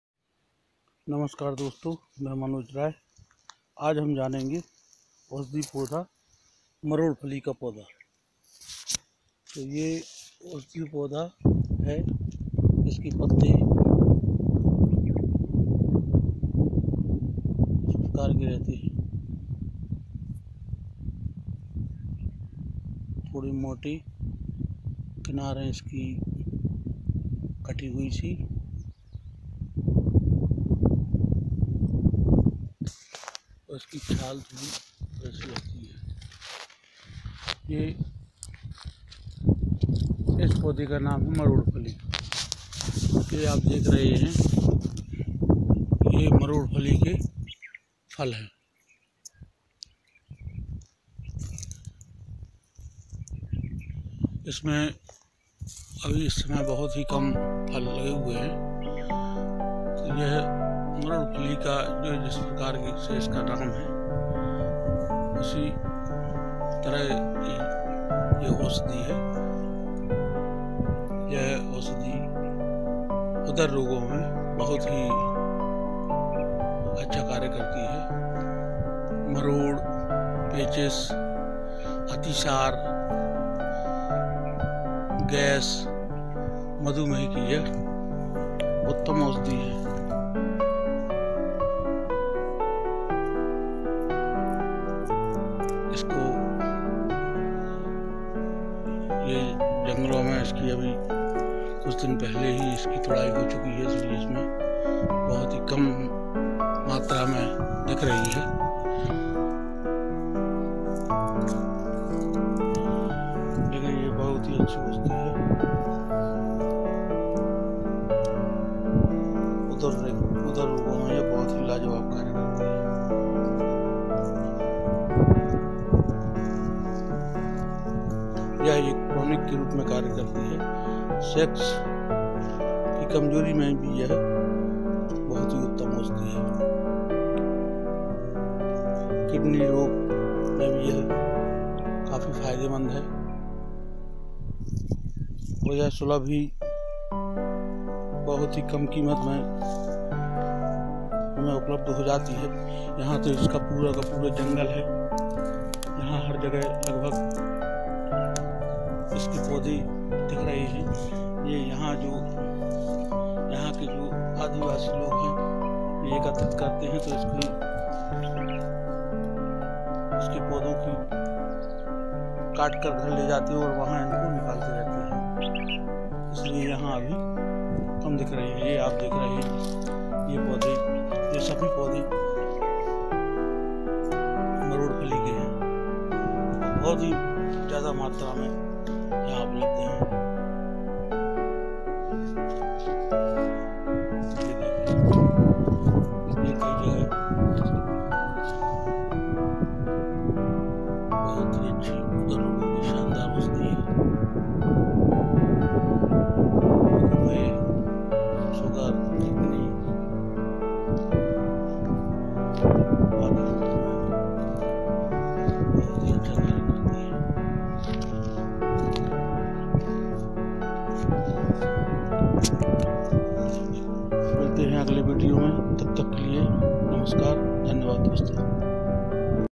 औषधीय पौधा मरोड़फली
औषधीय पौधा मरोड़फली, स्क्रू ट्री, आवर्तनी
इसे मरोड़फली, (एठनी,बैदी  भी कहते है)
उदर रोग, गैस, मरोड, पेचिस,अतिसार, मधुमेह,
की उत्तम औषधि है ।